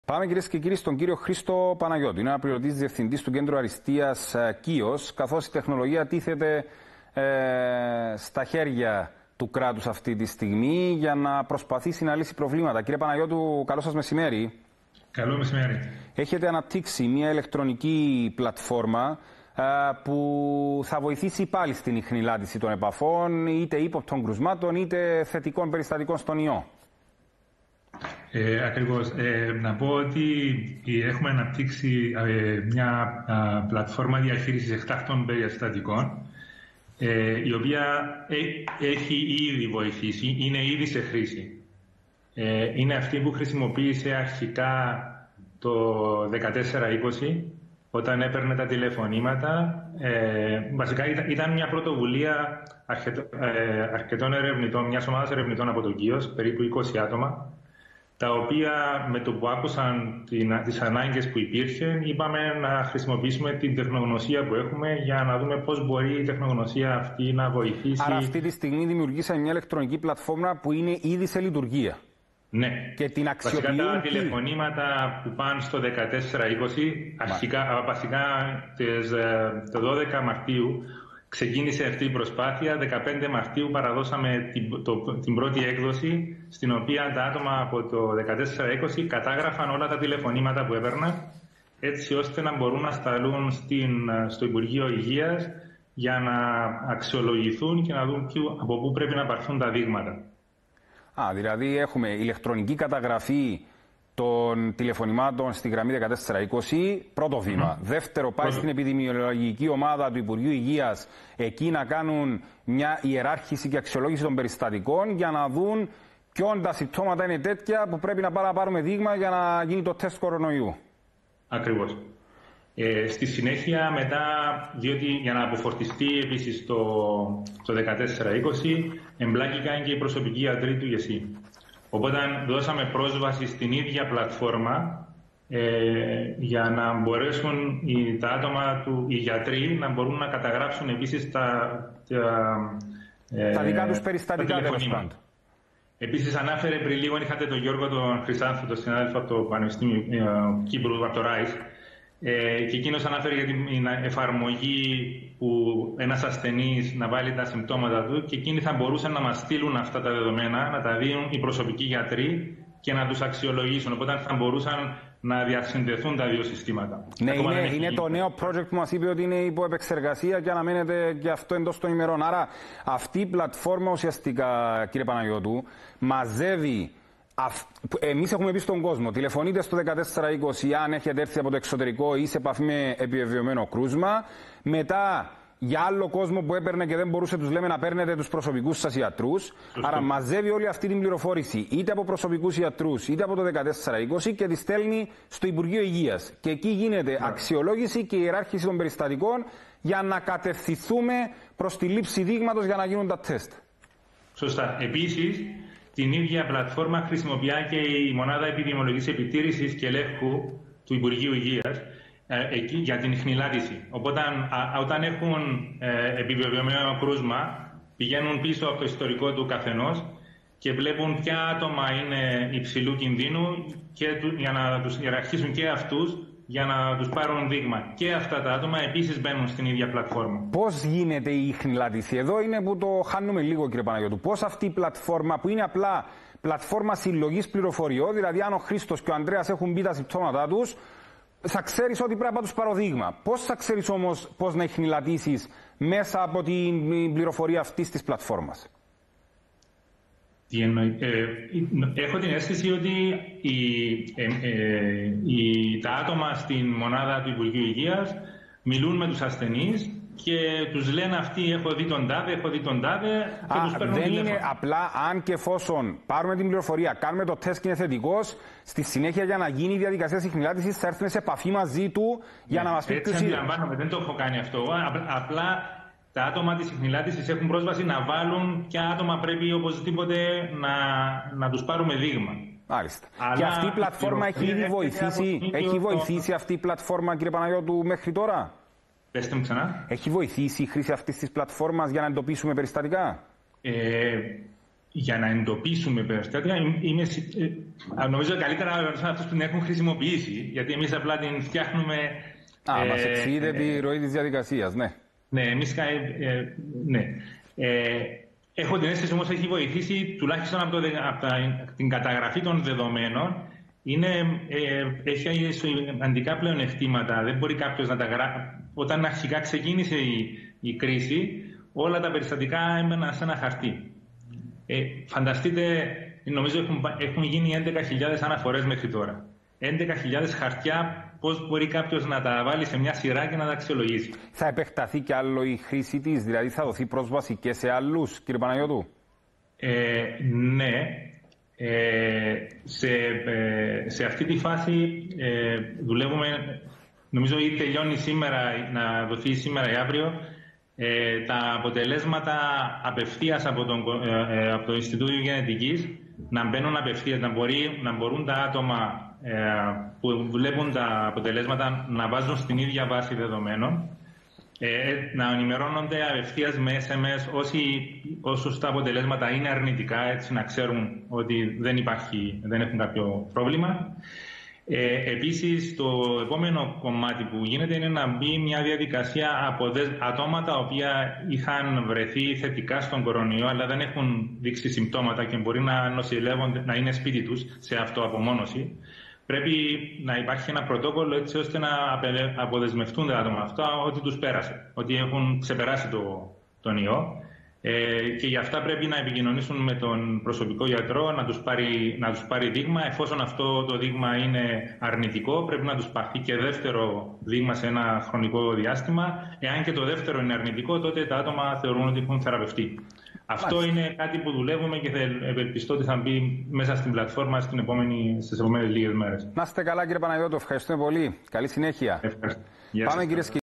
TV Presence of KIOS CoE Dep. Director Prof. Christos Panayiotou at the Alpha TV Channel.
TV Presence of KIOS CoE Dep. Director Prof. Christos Panayiotou at the Alpha TV Channel regarding the development of the KIOS ICT platform to assist the first responders in their efforts to prevent the spreading the COVID-19 in Cyprus.